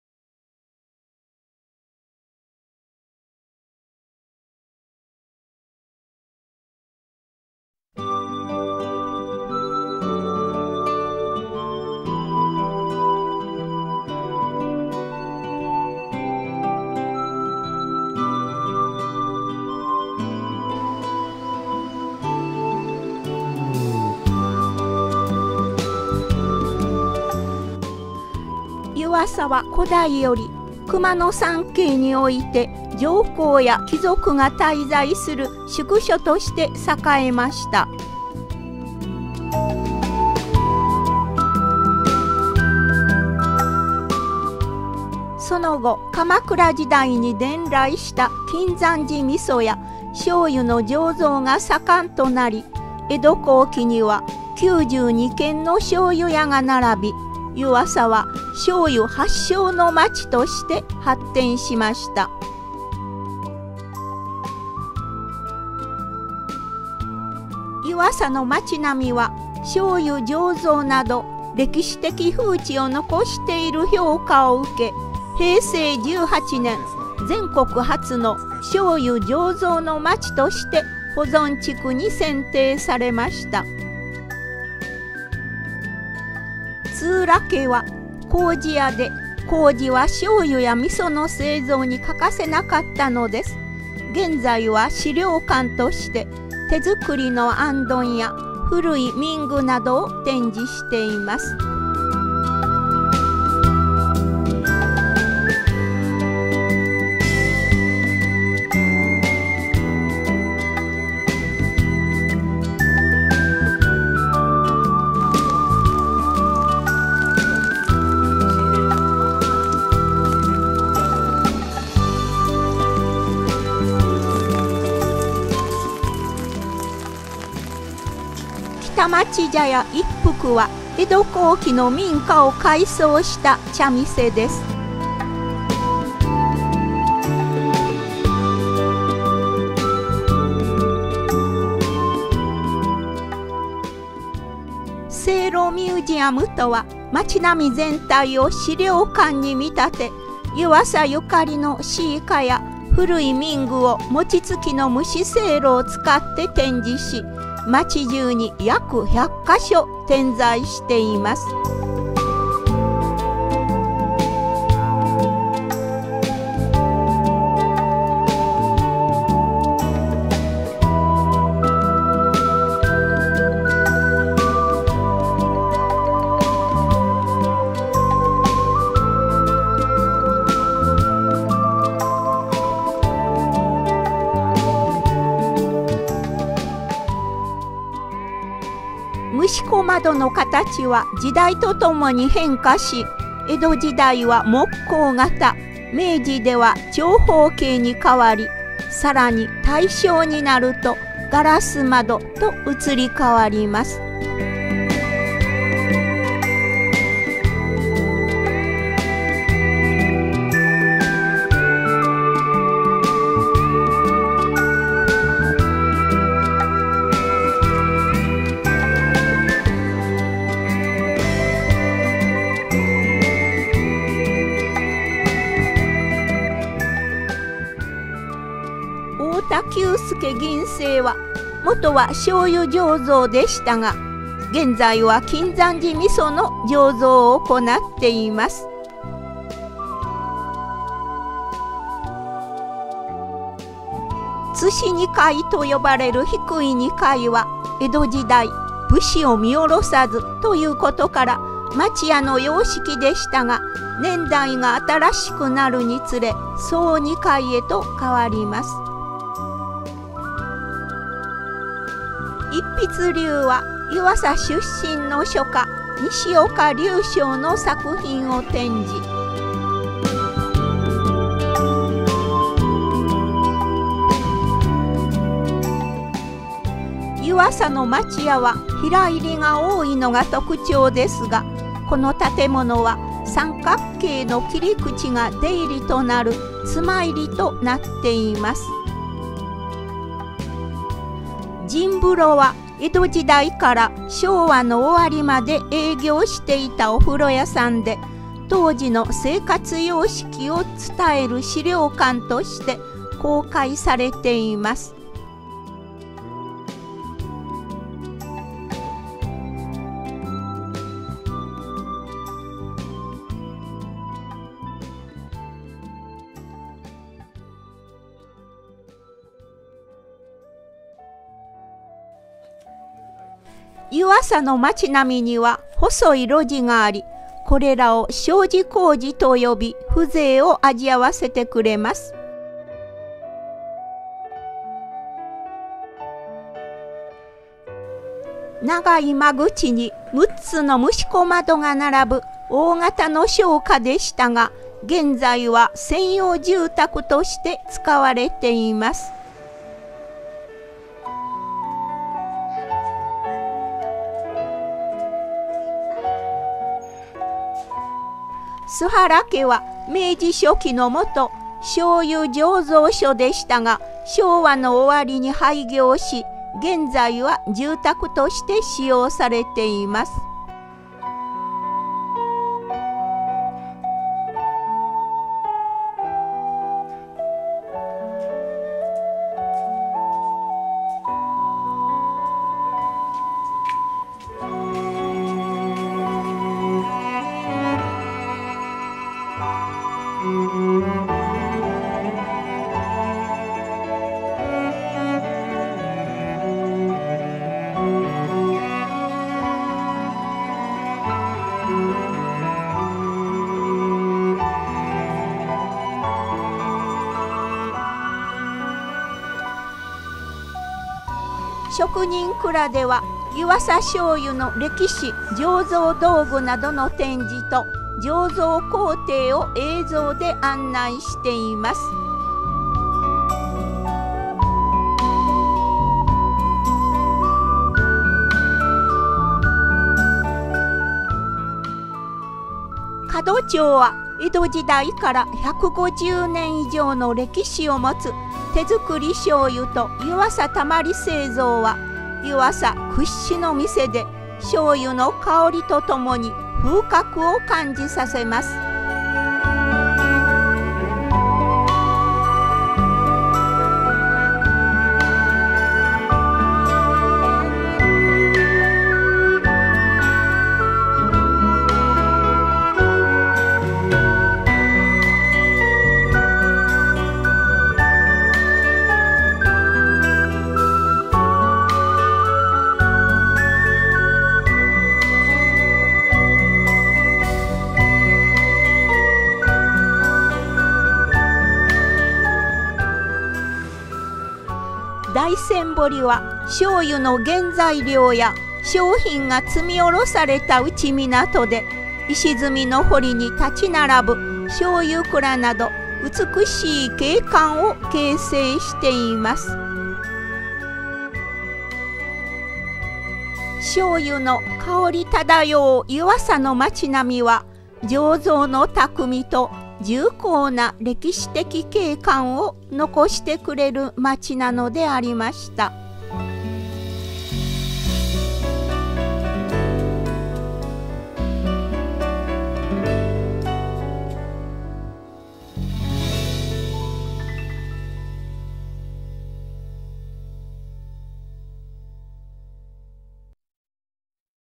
醤油の街・湯浅（ゆわさ）
和歌山県・湯浅は醤油醸造発祥の地です。醤油醸造文化が日本遺産に認定されています。又町並みは、伝統的建物保存地区に指定されています。